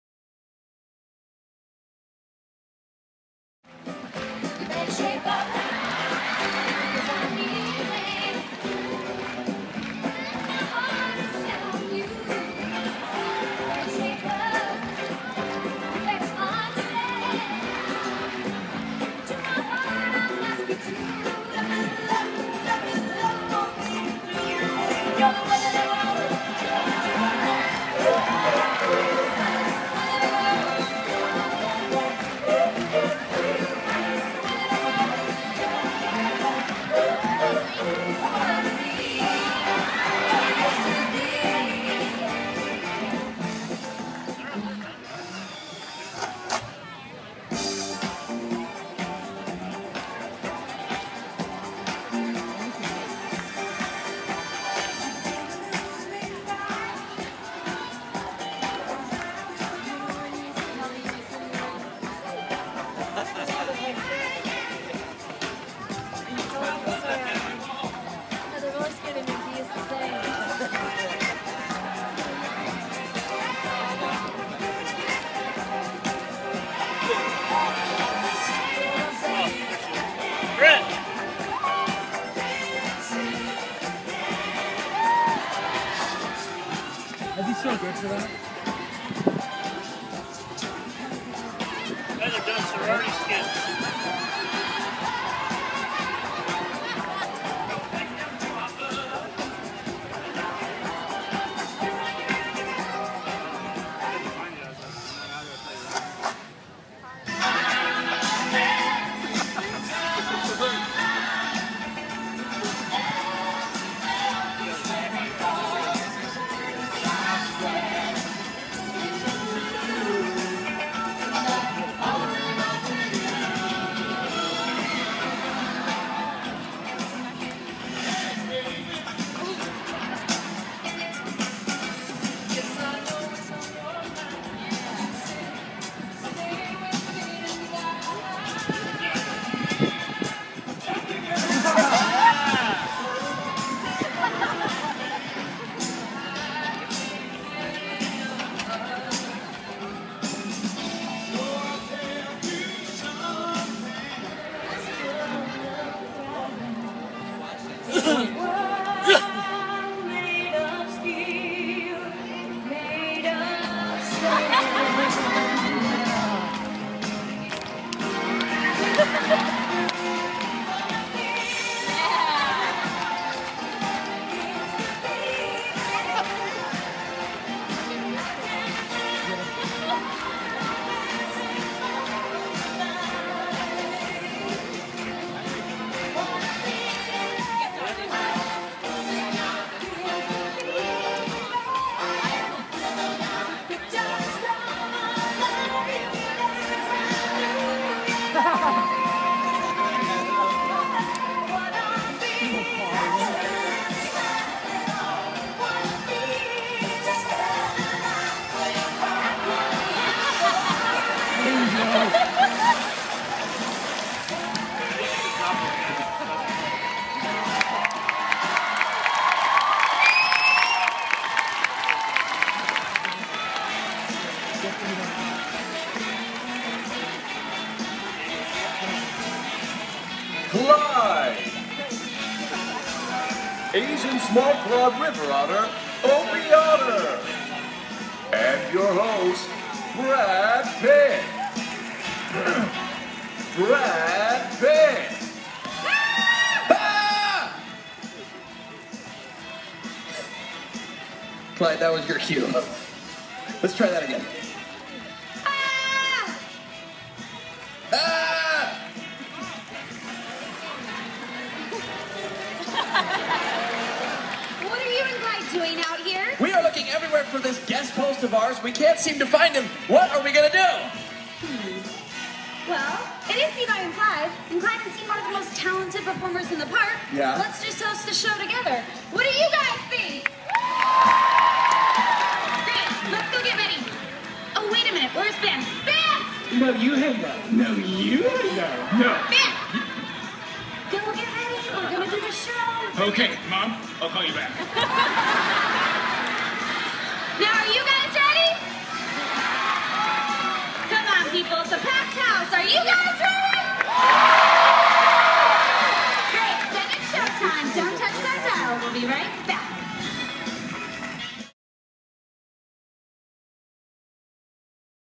Part 1 - A Very Entertaining Sea Lion Show @ Sea World, San Diego
I watched this during my USA trip to San Diego in March 2013. Very entertaining. Clever sea lions!